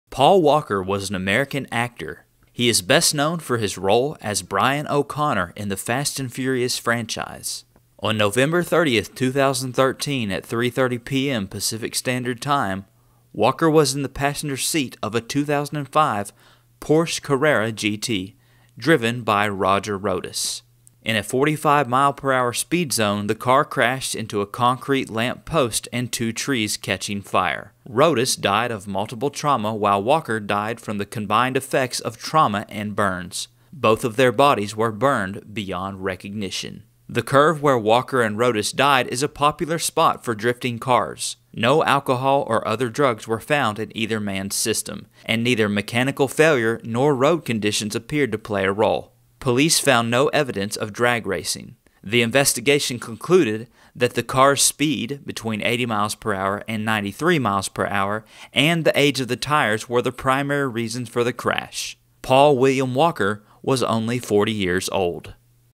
How Did Paul Walker Die?
Paul Walker was an American actor. He is best known for his role as Brian O'Conner in the Fast & Furious franchise.